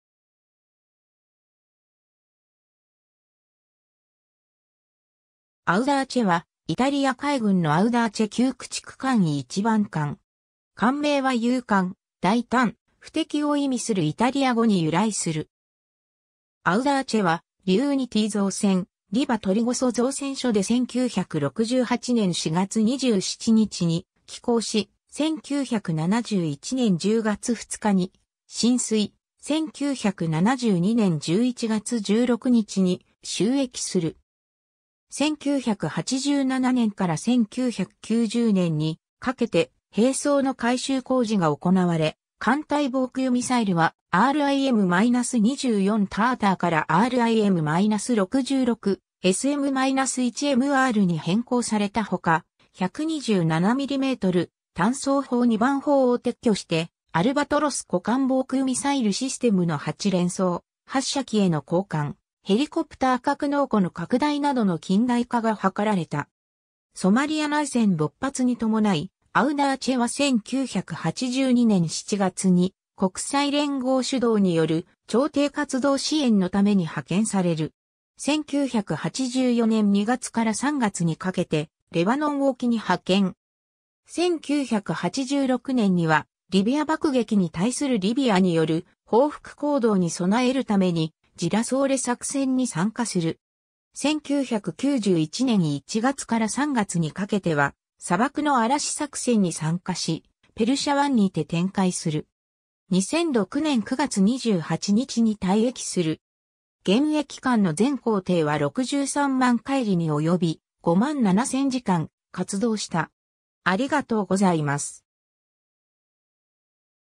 アウダーチェ (駆逐艦・3代)
アウダーチェ（イタリア語: Audace, D 551）は、イタリア海軍のアウダーチェ級駆逐艦1番艦。
艦名は勇敢、大胆、不敵を意味するイタリア語に由来する。
 「アウダーチェ」は、リウーニティ造船リヴァ・トリゴソ造船所で1968年4月27日に起工し、1971年10月2日に進水、1972年11月16日に就役する。
 1987年から1990年にかけて兵装の改修工事が行なわれ、艦隊防空ミサイルはRIM-24 ターターからRIM-66 SM-1MRに変更されたほか、127mm単装砲2番砲を撤去してアルバトロス個艦防空ミサイル・システムの8連装発射機への交換、ヘリコプター格納庫の拡大などの近代化が図られた。
 ソマリア内戦勃発に伴い「アウダーチェ」は1982年7月に国際連合主導による調停活動支援のために派遣される。
1984年2月から3月にかけてレバノン沖に派遣。
1986年にはリビア爆撃に対するリビアによる報復行動に備えるためにジラソーレ作戦に参加する。
1991年1月から3月にかけては砂漠の嵐作戦に参加しペルシャ湾にて展開する。
 2006年9月28日に退役する。
現役間の全行程は630,000海里におよび、57,000時間活動した。